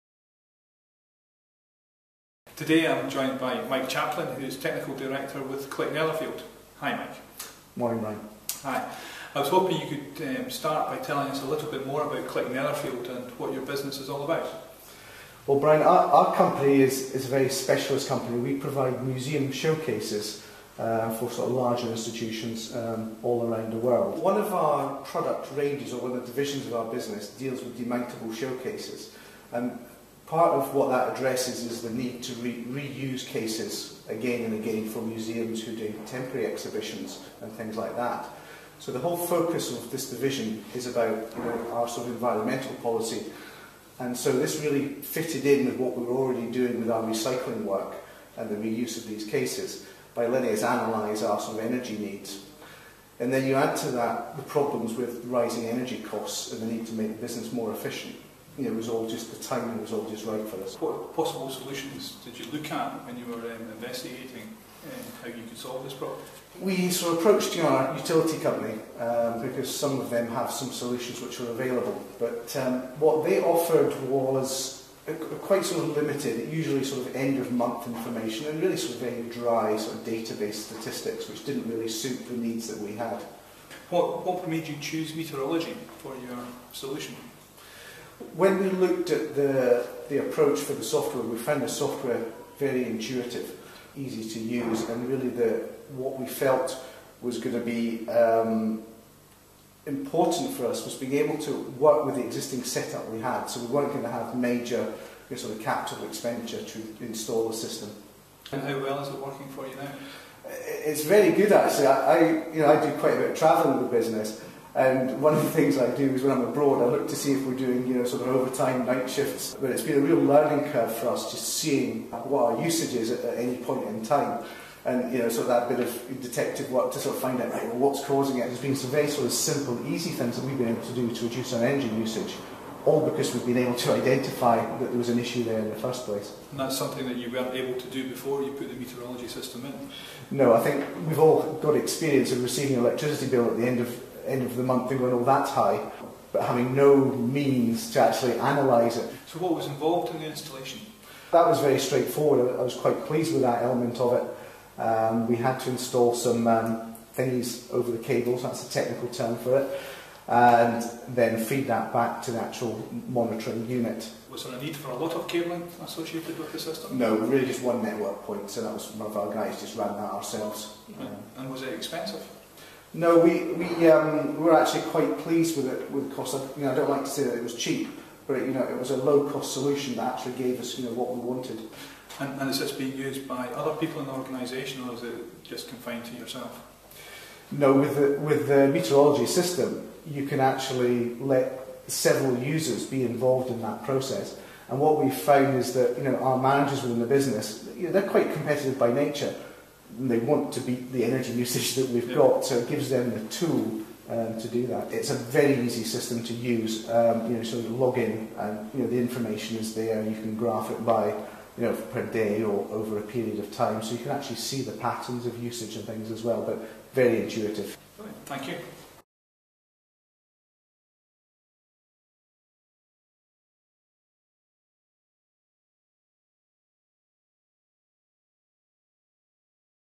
Mike Chaplin of ClickNetherfield Interview on Meterology Solution
Mike Chaplin of ClickNetherfield is interviewed on the Meterology electricity use analysis solution.